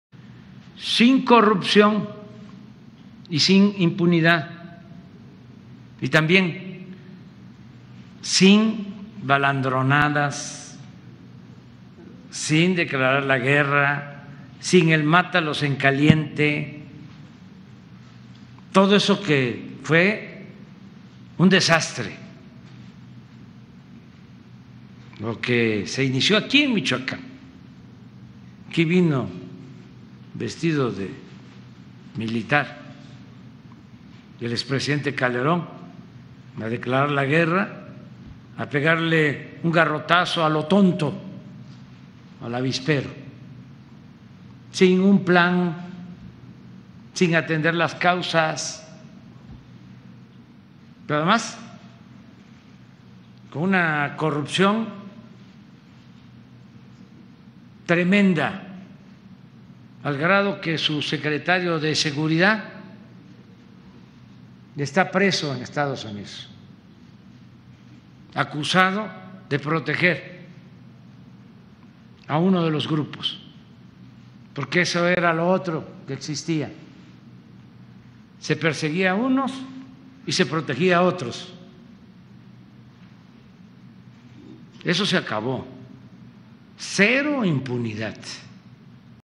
A Michoacán vino Felipe Calderón vestido de militar a pegarle a lo tonto garrotazo a avispero: AMLO
Aquí a Michoacán vino Felipe Calderón vestido de militar a "pegarle a lo tonto un garrotazo al avispero": AMLO. Lamentó la corrupción tremenda en el sexenio calderonista, incluyendo el caso de García Luna quien protegió a delincuentes.